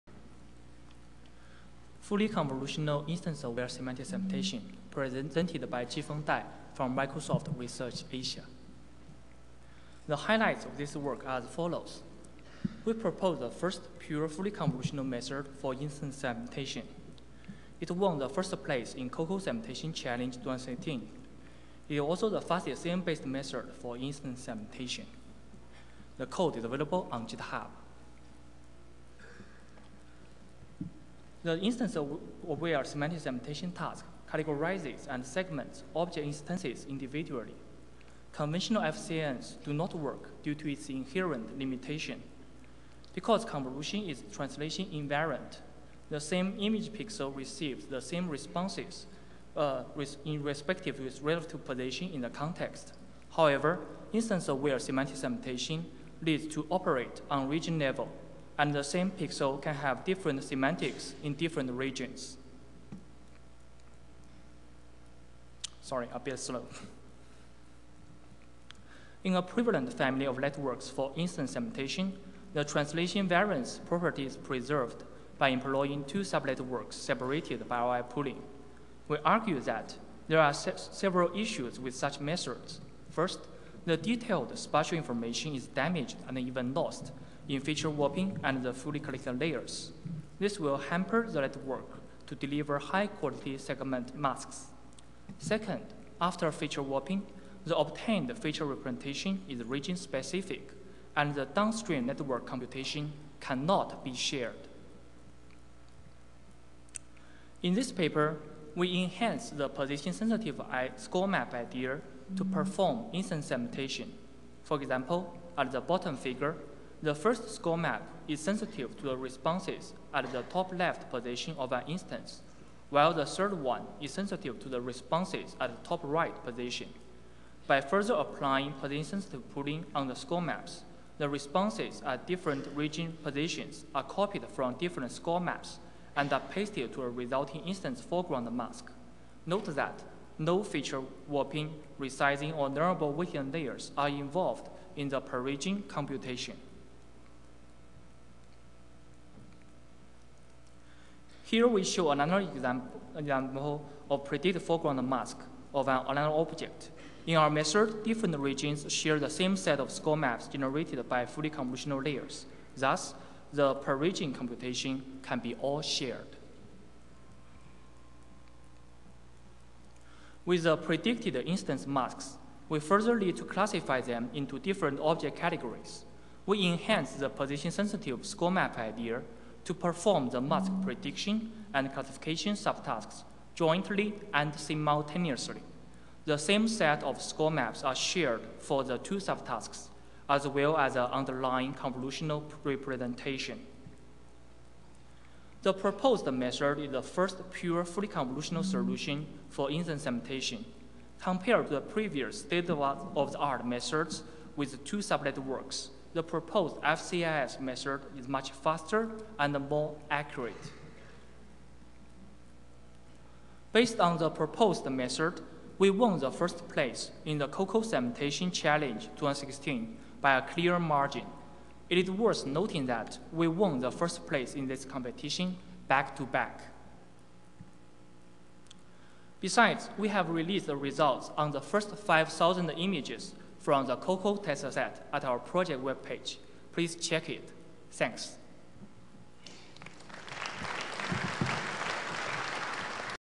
Fully Convolutional Instance-Aware Semantic Segmentation | Spotlight 3-1B
Yi Li; Haozhi Qi; Jifeng Dai; Xiangyang Ji; Yichen Wei
We present the first fully convolutional end-to-end solution for instance-aware semantic segmentation task. It inherits all the merits of FCNs for semantic segmentation and instance mask proposal. It performs instance mask prediction and classification jointly. The underlying convolutional representation is fully shared between the two sub-tasks, as well as between all regions of interest. The network architecture is highly integrated and efficient. It achieves state-of-the-art performance in both accuracy and efficiency. It wins the COCO 2016 segmentation competition by a large margin. Code would be released.